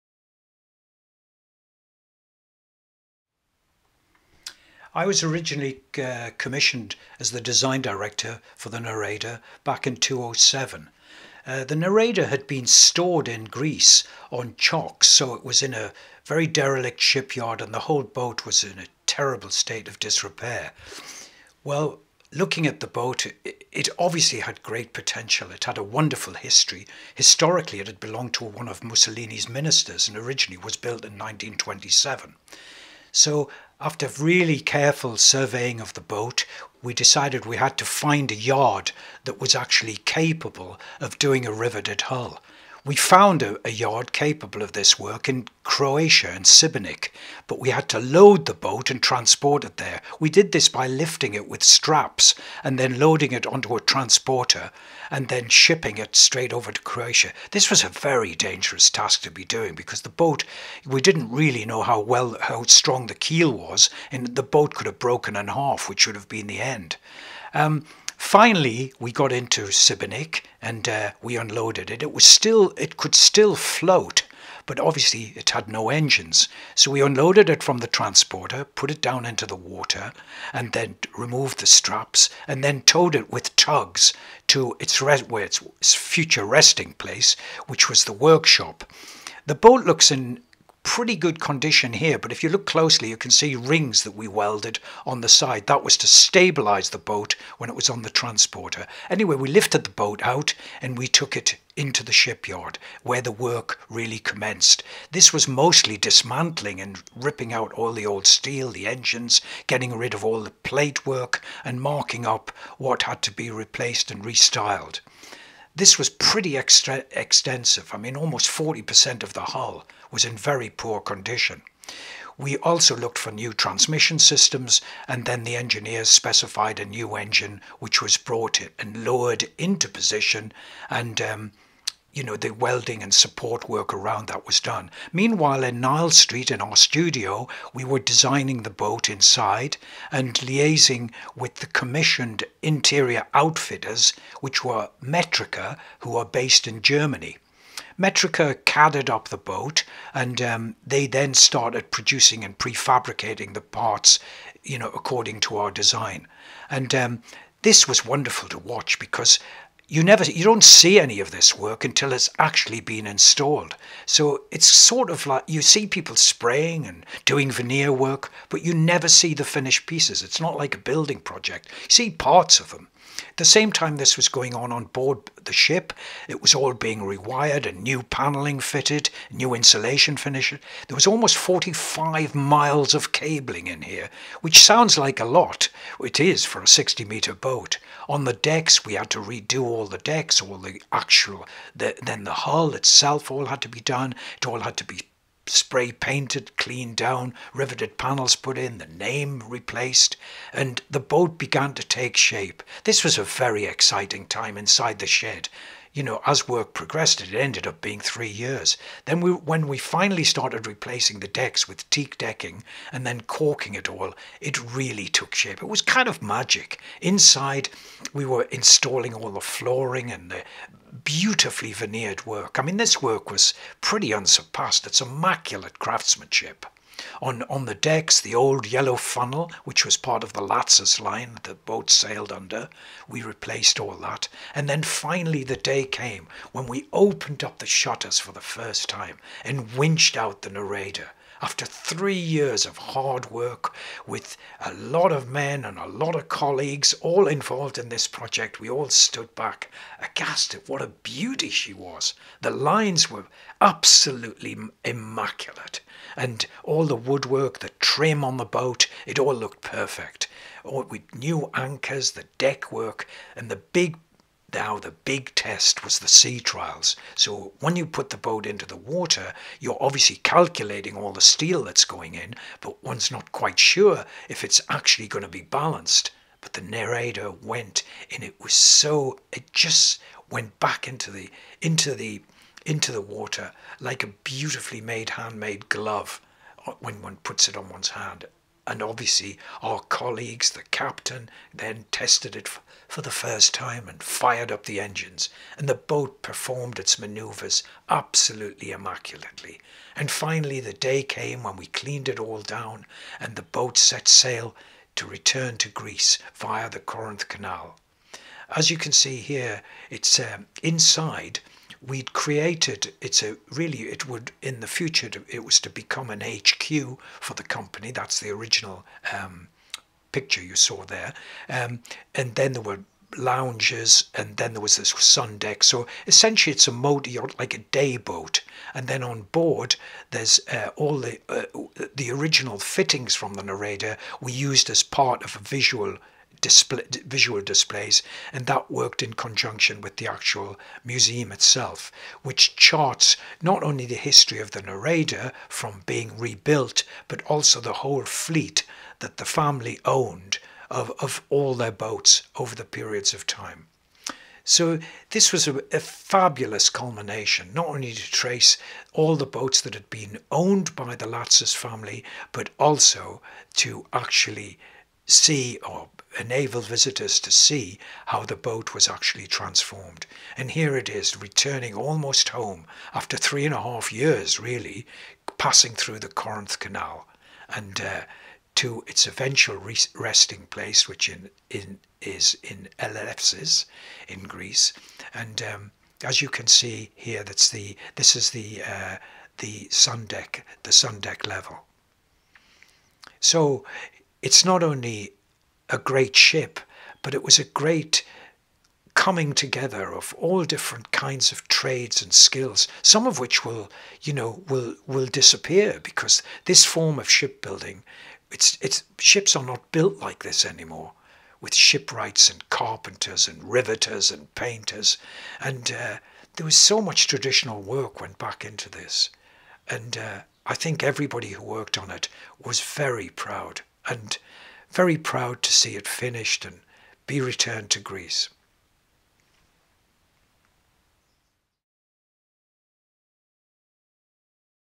Doug Patterson. Reconstruction of the Neraida
THE NERAIDA
Doug Patterson was commissioned as the design director to totally rebuild  the Neraida motor yacht which had been stored in dry dock for more than 20 years in Elefsis Greece. The NCP shipyard in Sibenick in Croatia was commissioned to rebuild the 60 metre vessel and to then transport her by sea from Greece to the Croatia. The rebuild commenced  which took three years. In tandem, the interior outfitting was undertaken by Metrica  who were based in Sedden in Germany. All the interior components were transported in 40 foot containers by road and installed by their master craftsmen. The Neraida was finally relaunched and returned triumphantly to Greece where she is based and is currently a private museum .

CREDITS           FILM DIRECTOR                   RAMSAY CAMERON
                           FILM EDITOR                       ANNIE OWEN
                           SHIPBUILDERS                    NCP SIBENIK   CROATIA
                           INTERIOR OUTFITTING      METRICA SEDDEN GERMANY